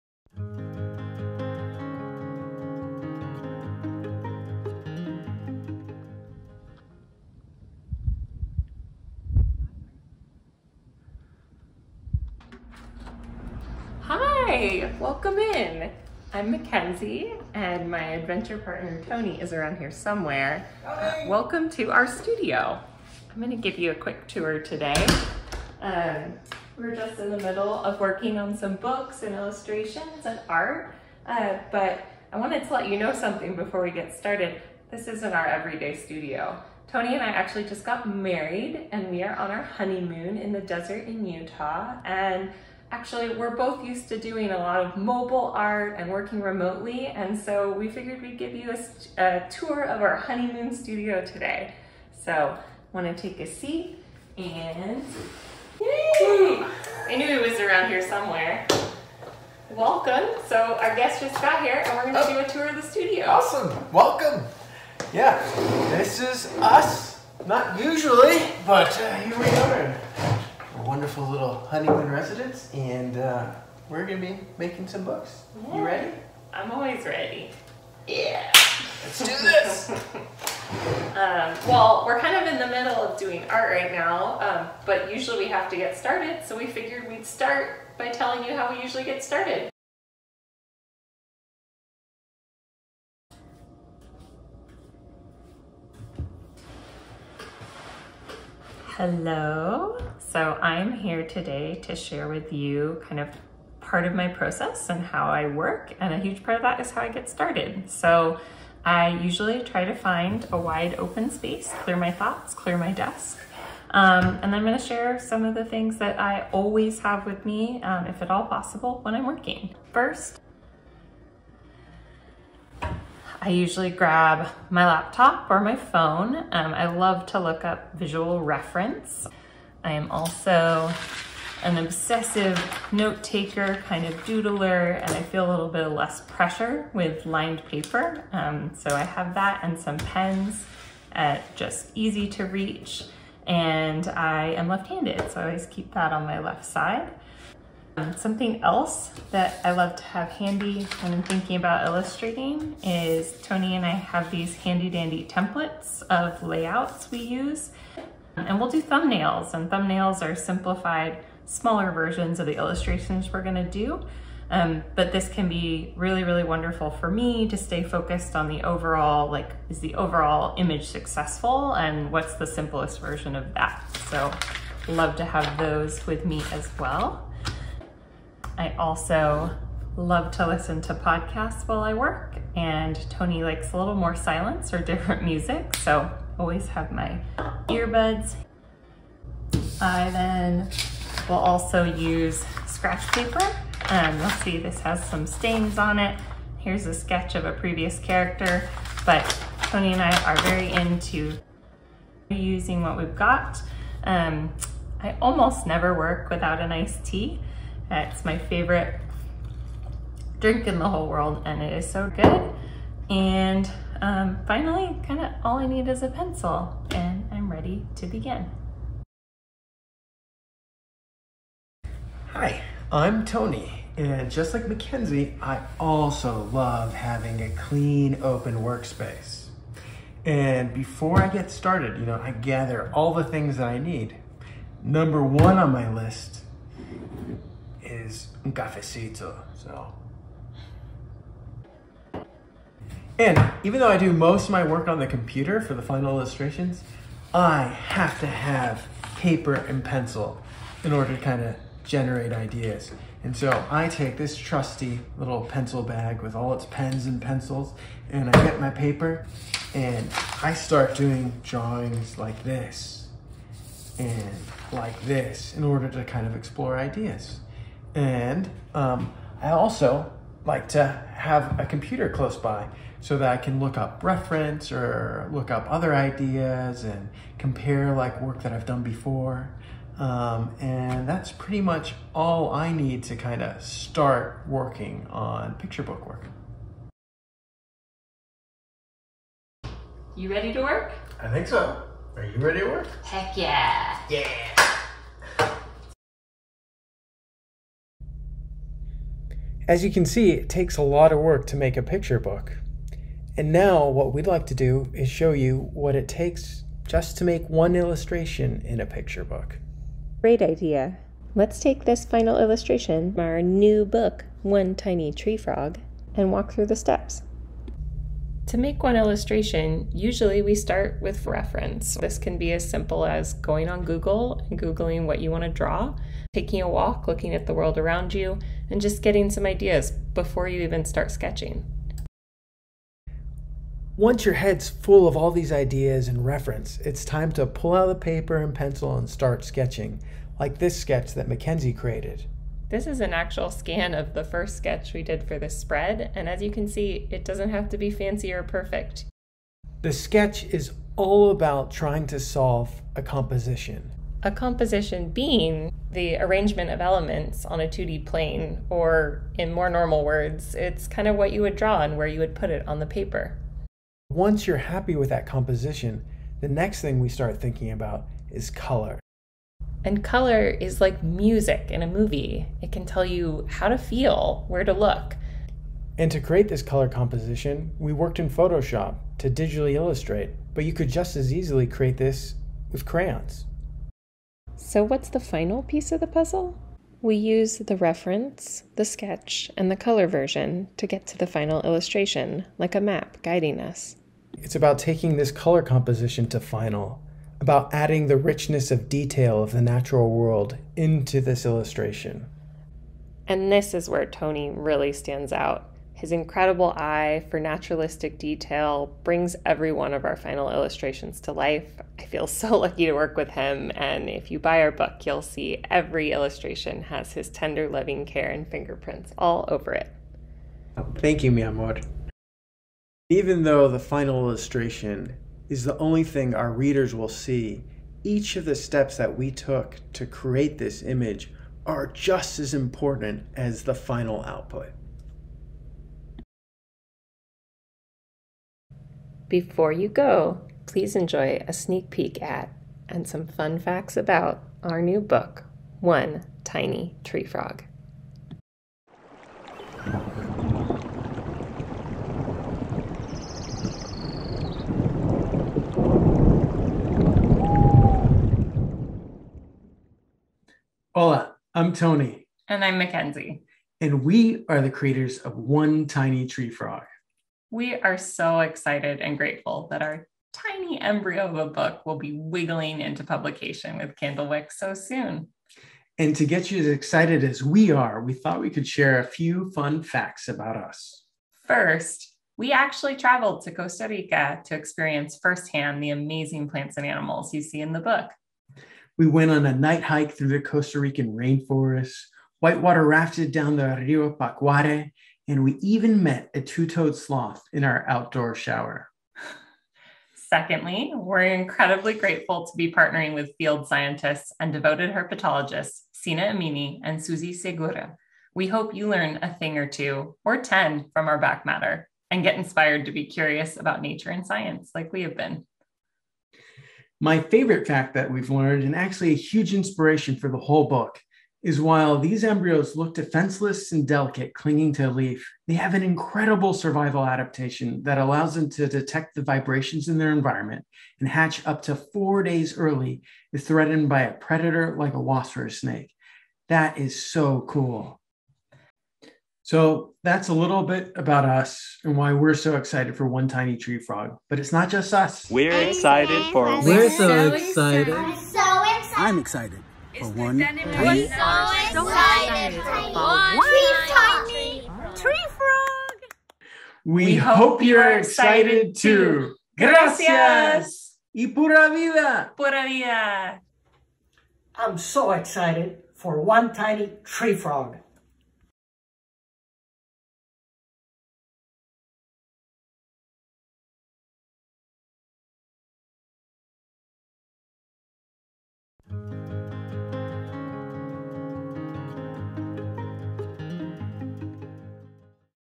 Candlewick Open Studios: Tony Piedra and Mackenzie Joy
Learn about Tony Piedra and Mackenzie Joy's illustration process

About the Book:
One Tiny Treefrog: A Countdown to Survival
Tony Piedra and Mackenzie Joy

Deep in the tropical forest of Costa Rica, ten sticky frog eggs cling to a leaf. Only nine eggs hatch. Only eight tadpoles wind up in the water below. What will it take to survive the countdown to adulthood and begin the cycle again? This beautifully designed, color-drenched introduction to frog metamorphosis peeks at this animal’s habitats and predators as it illustrates competition and natural selection. Cocreators Mackenzie Joy and Tony Piedra have crafted a dazzling spin on the life of one of nature’s darlings, the red-eyed treefrog. Back matter invites budding naturalists to discover even more treefrog facts, revisit the pages to spot other Costa Rican species, and check out a bibliography.